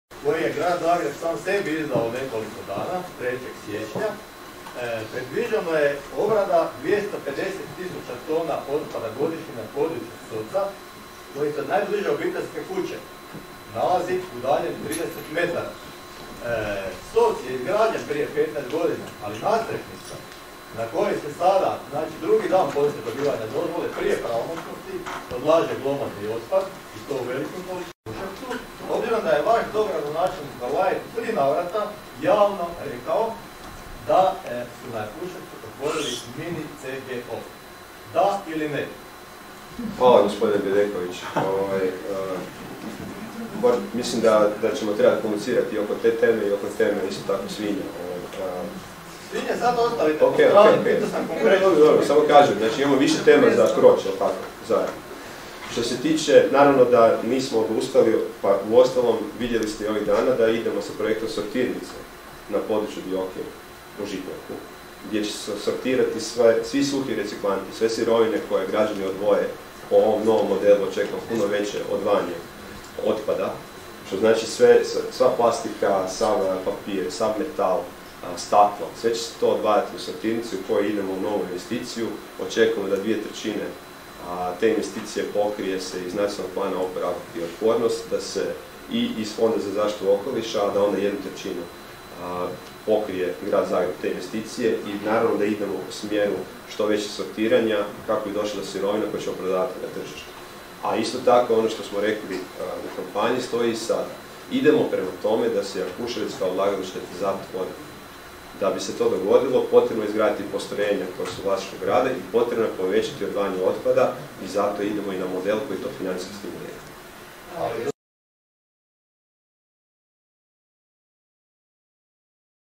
Gradonačelnik Tomašević o projektu sortirnice otpada
Gradonačelnik Grada Zagreba Tomislav Tomašević, sa suradnicima, predstavio je web aplikaciju Grada Zagreba Mentalno zdravlje. Riječ je o važnom projektu koji će služiti kao središnje mjesto informiranja građana o mentalnom zdravlju s obzirom na to da će ovim web portalom na jednom mjestu biti objedinjeni svi projekti Grada Zagreba i gradskih zdravstvenih ustanova u području zaštite mentalnog zdravlja, koji će na ovaj način postati vidljiviji i dostupniji za šire skupine građana.